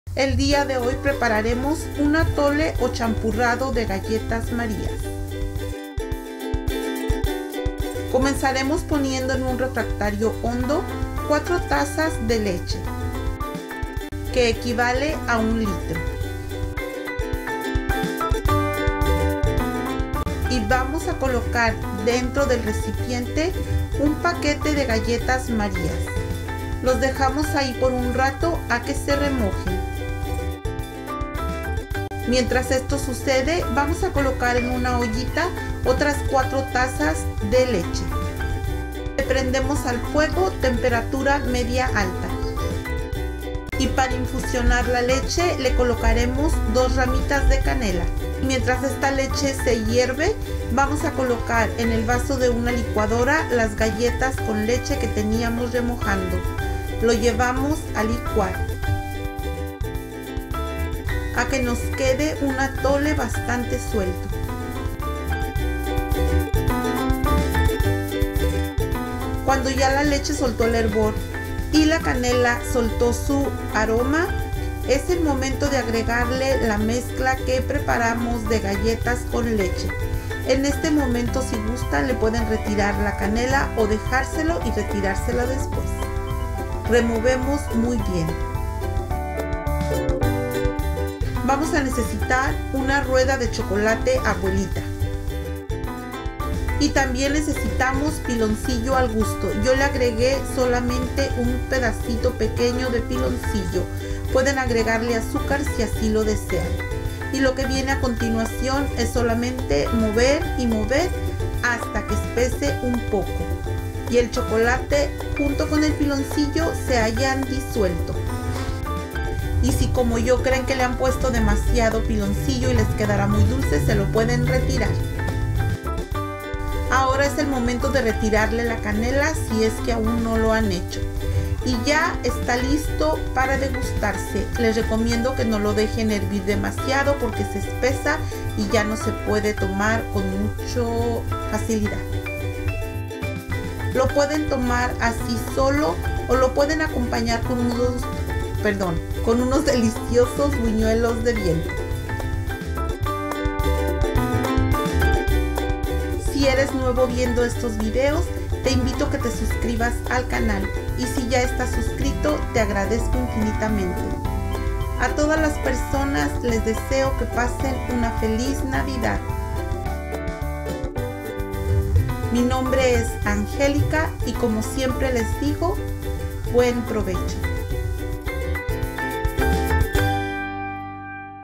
ATOLE O CHAMPURRADO DE GALLETAS MARIA'S
Para este CHAMPURRADO (ATOLE) DE GALLETAS MARIA'S, necesitas:
8 tazas de leche, dividida,
1 paquete de galletas Maria's (140 gramos),
2 rajitas de canela,
1 rueda de chocolate Abuelita, Ibarra, o el que consigas de mesa,
piloncillo o azúcar al gusto.

FELIZ NAVIDAD, les desea ANGELICA QUEZADA de SAZONANDO CON AMOR.